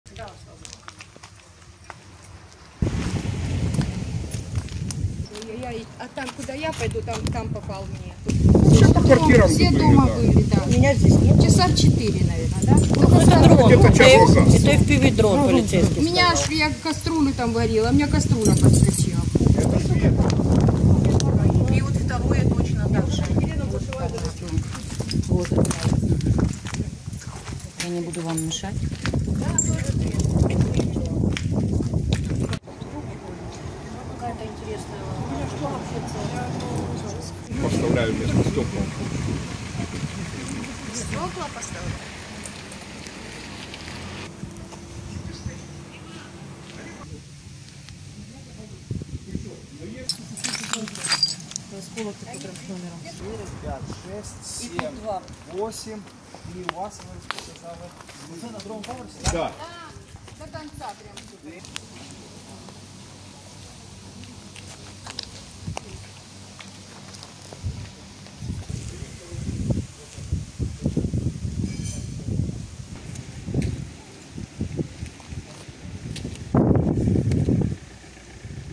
Обстріл центру Херсона: російська армія атакувала стару частину міста
Вулиця Робоча. Херсон.Жваве  місце,  через яке багато хто біжить на ринок або полюбляє купити води питної. Тепер міська  влада показує після обстрілу. Місцеві жителі показують, як виглядають їх оселі. Удар був сильним.Вибиті вікна, потрощені автівки, налякані люди...

Удар був близько 16.30 години по центру міста.

На місце одразу прибули представники Херсонської міської військової адміністрації, щоб з'ясувати, яка  допомога їм потрібна. Відео- Херсонська МВА.